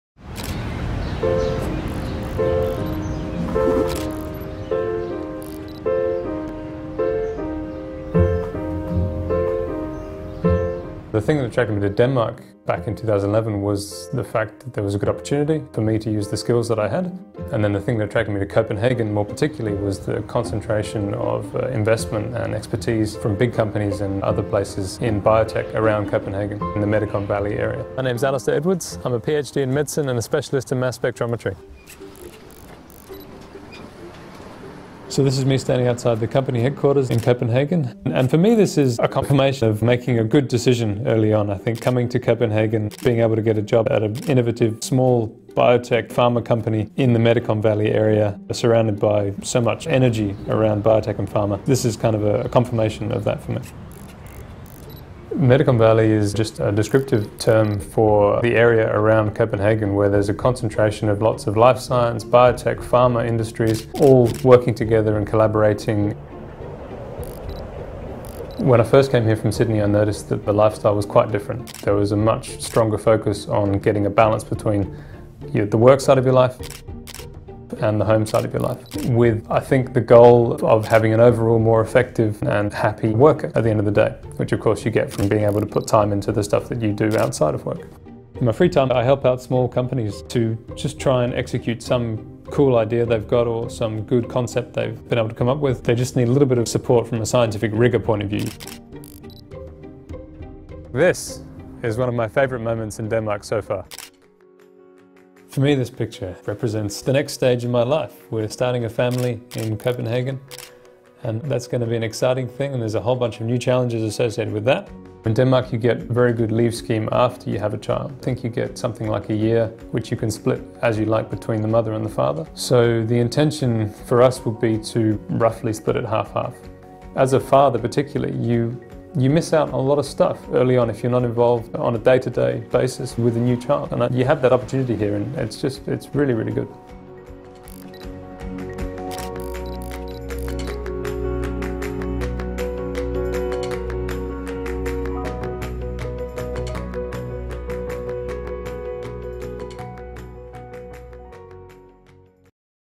My Big Danish Moment - Alistair from Australia
Meet Alistair from Australia. He is a specialist in mass spectrometry. He works in a biotech company in Copenhagen. He chose Denmark because of the vibrant research/business scene in life science in Medicon Valley located in Greater Copenhagen and Southern Sweden, cool lifestyle and work-life balance.